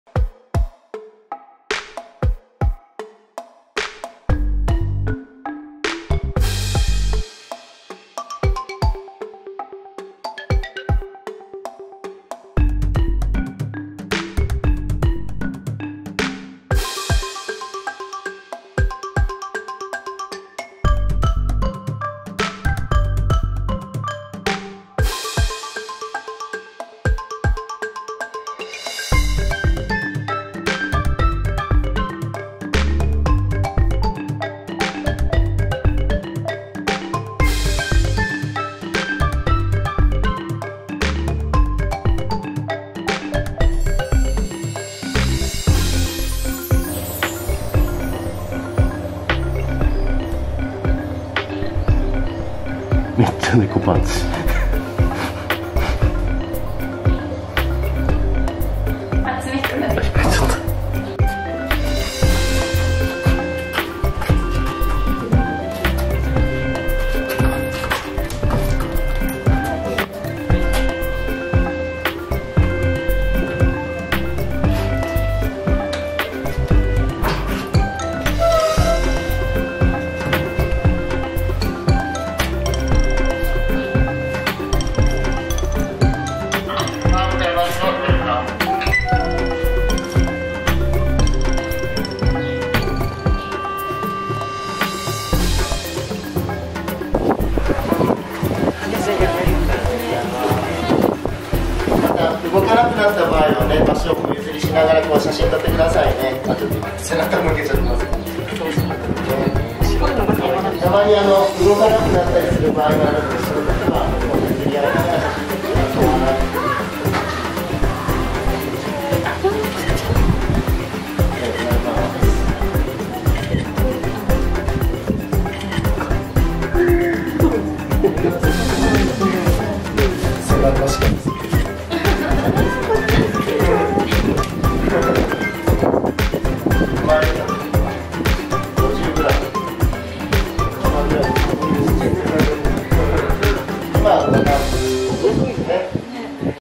お目々ぱっちり活発になってきたスナネコの赤ちゃん
2020年4月27日に那須どうぶつ王国で生まれた世界最小級ネコ、スナネコの赤ちゃん。一般公開されてみんなと会えるようになりました！

▼チコちゃんと那須どうぶつ王国コラボグッズ販売中です✨
▼ダーウィンが来た！みんなのどうぶつえんすいぞくかん